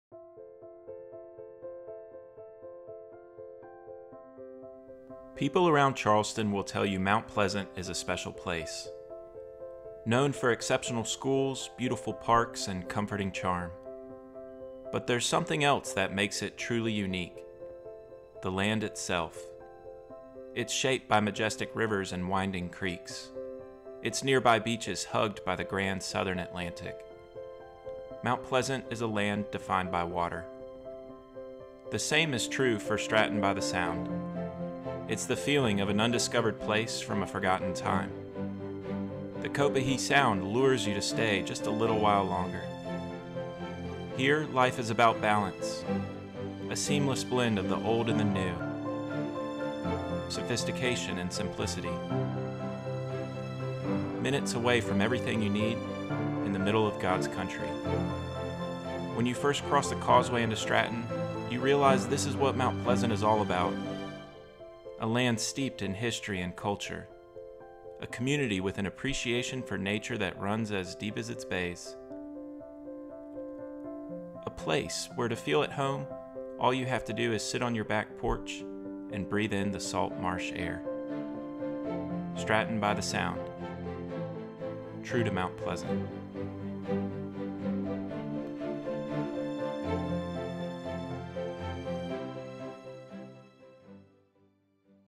Stratton by the Sound, an Ashton Woods Community in Charleston, SC
In the master-planned community of Stratton by the Sound, lowcountry Charleston-style homes share inspiring views of Dewees Island, Copahee Sound and the Atlantic Ocean. Neighbors gather at the community crabbing dock, under the shade of a pool cabana or at the playground. Here, together, we'll create your home as a true reflection of your style and passions. Whether inspired by the old world charm of Charleston or the natural beauty of your community, at Stratton by the Sound, you'll love the possibilities.